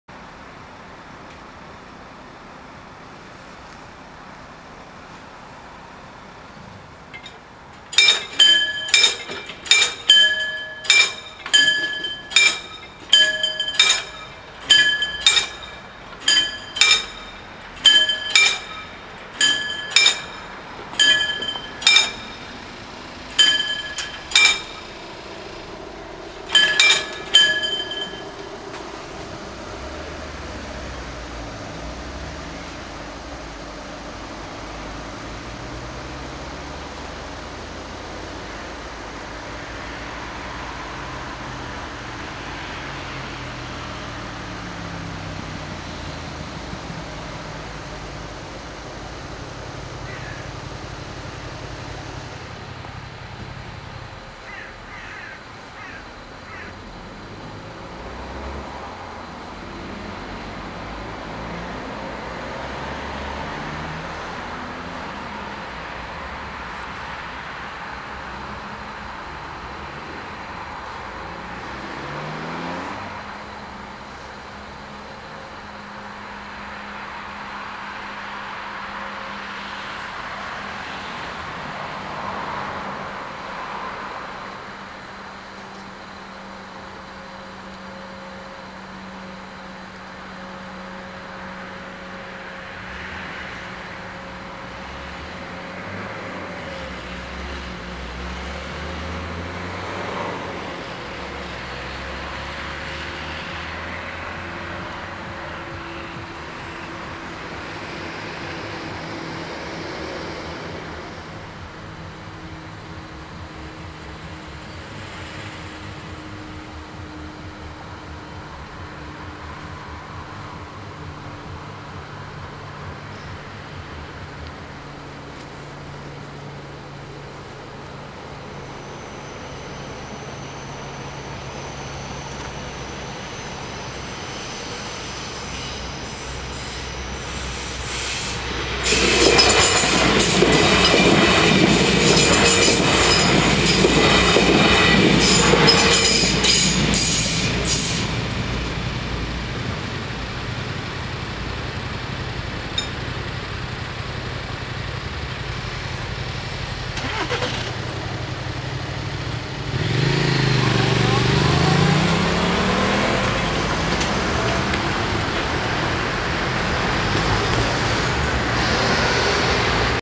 Spoorwegovergang Braunschweig (D) // Railroad crossing // Bahnübergang
Locatie: Wendebrück
Traject: Braunschweig - Gifhorn
Ontruimingslichten: 5
Bellen: 2
Bomen: 2
Andreaskruisen: 5
Passeren:
- LINT 54 als RB47 Braunschweig → Uelzen